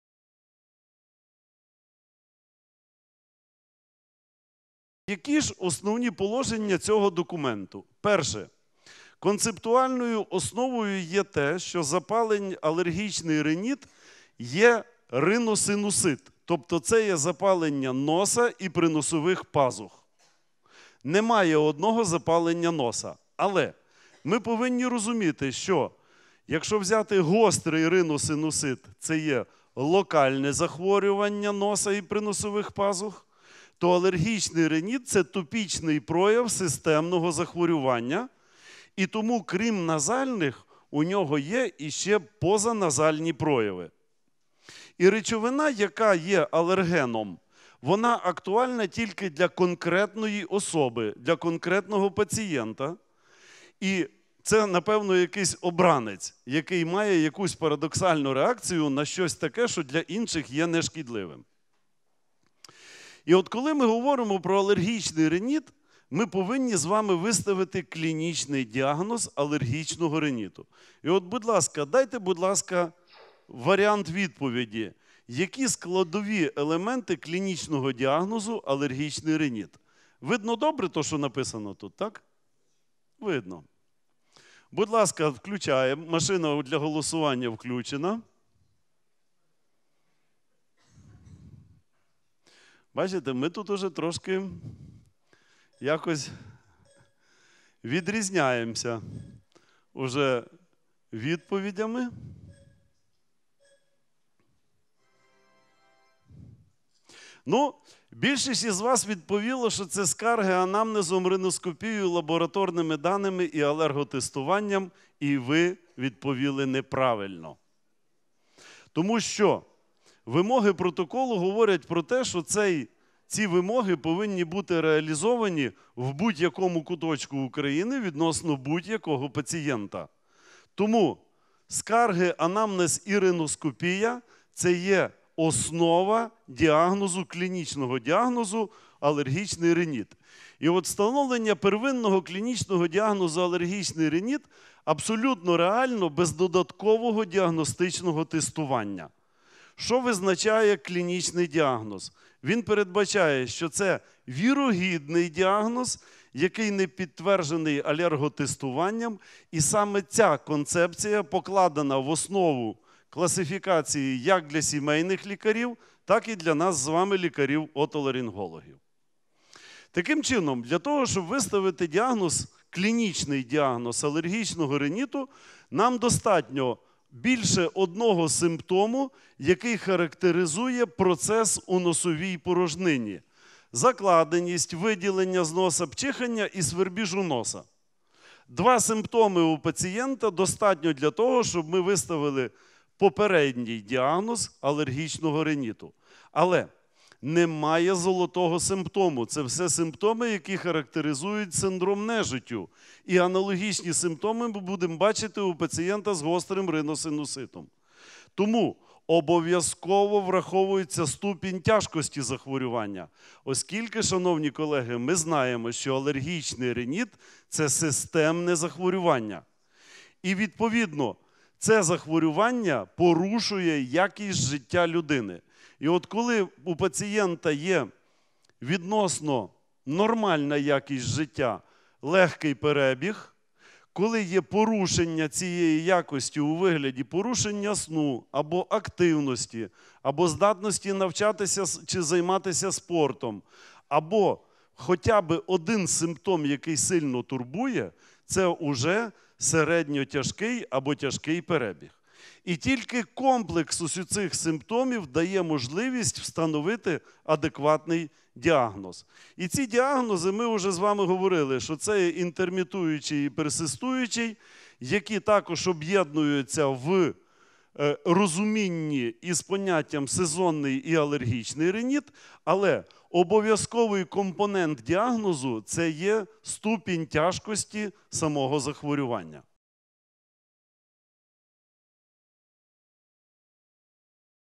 Основні положення нормативно-правової бази діагностики алергічного риніту
Основні положення настанови "Алергічний риніт". Визначення алергічного риніту як запалення слизової оболонки та приносових пазух. Складові елементи клінічного діагнозу алергічного риніту. Встановлення первинного клінічного діагнозу АР без додаткового діагностичного тестування. Клінічні симптоми захворювання. Класифікація алергічного риніту: інтермітуючий та персистуючий.

Коментує голова експертної групи МОЗ України за напрямом "Отоларингологія. Дитяча отоларингологія. Сурдологія", доктор медичних наук, професор Попович Василь Іванович

Основные положения руководства "Аллергический ринит". Определение аллергического ринита как воспаления слизистой оболочки и приносовых пазух. Составляющие элементы клинического диагноза АР без дополнительного диагностического тестирования. Клинические симптомы заболевания. Классификация аллергического ринита: интермитирующий и персистирующий.

Комментирует глава экспертной группы МОЗ Украины по направлению «Отоларингология. Детская отоларингология. Сурдология», доктор медицинских наук, проф. Попович Василий Иванович